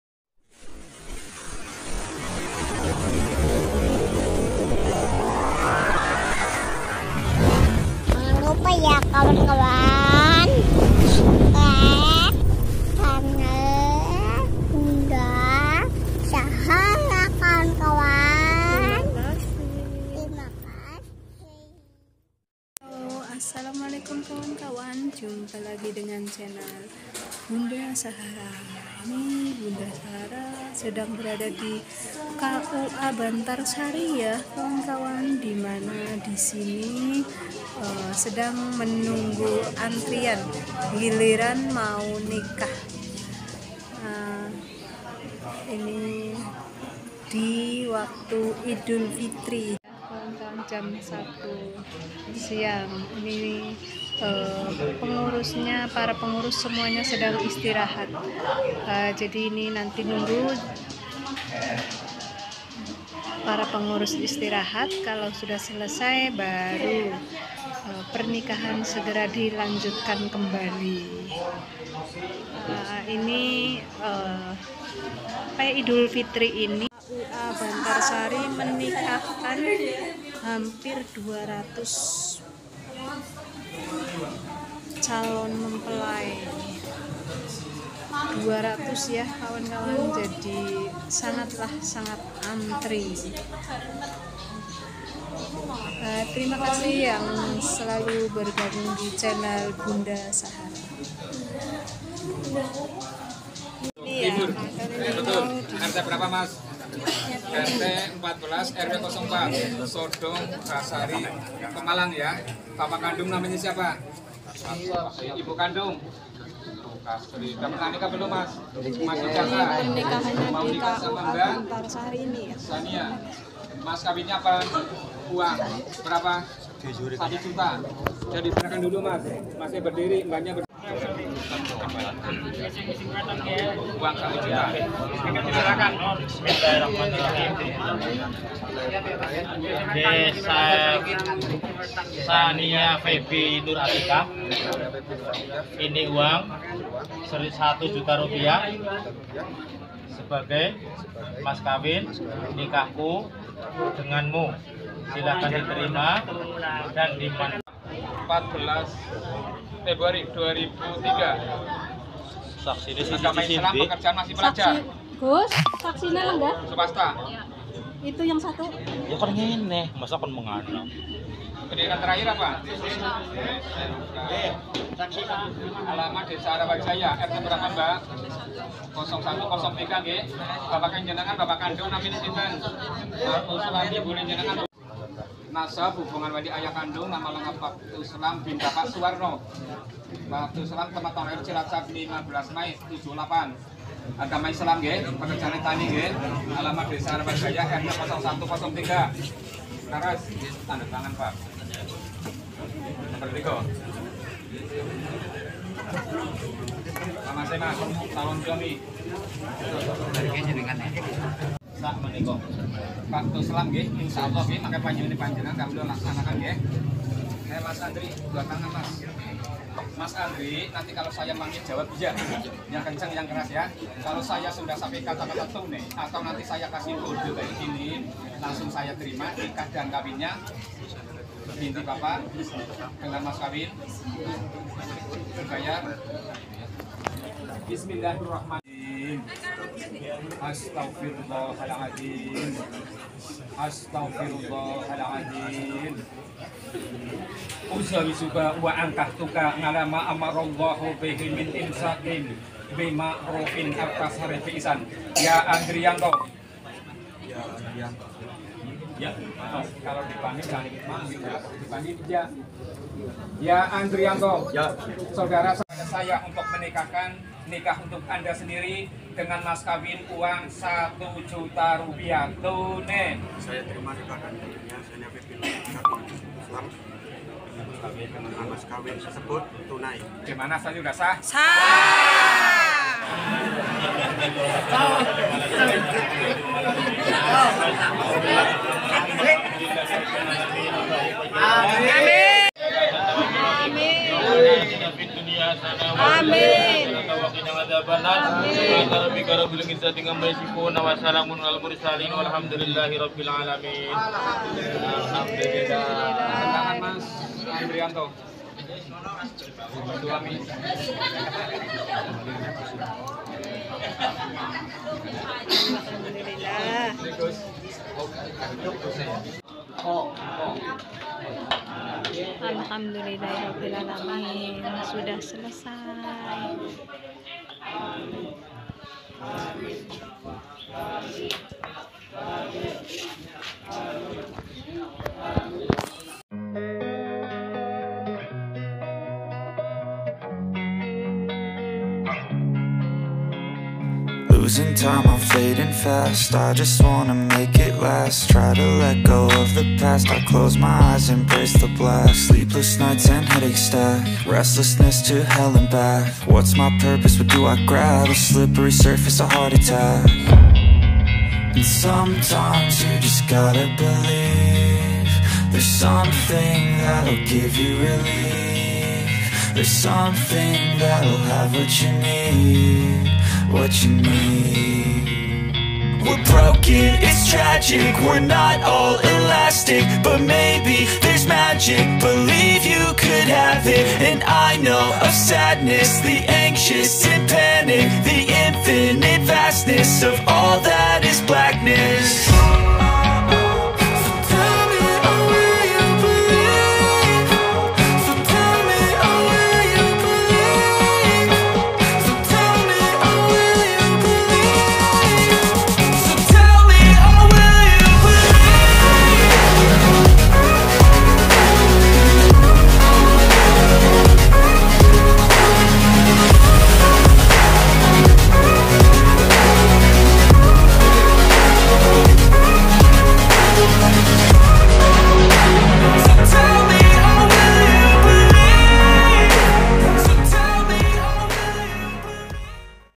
Acara Akad Nikah di Hari Raya Idul fitri
Acara Pernikahan di Hari Raya Idul fitri semoga SAMAWA Aamiin, Mohon do'a restonya sedulur semua